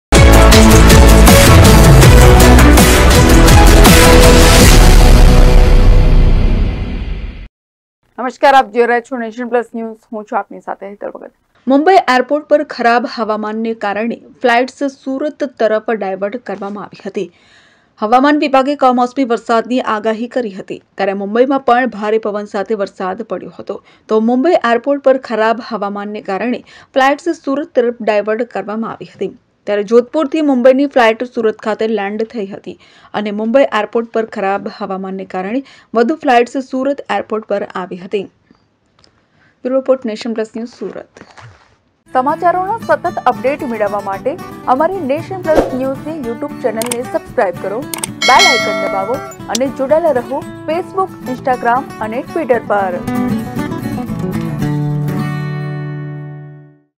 ફ્લાઈટ્સ સુરત એરપોર્ટ પર ડાયવર્ટ કરાઇ
મુંબઈ એરપોર્ટ પર ખરાબ હવામાનને કારણે ફ્લાઈટ્સ સુરત તરફ ડાયવર્ટ કરવામાં આવી હતી.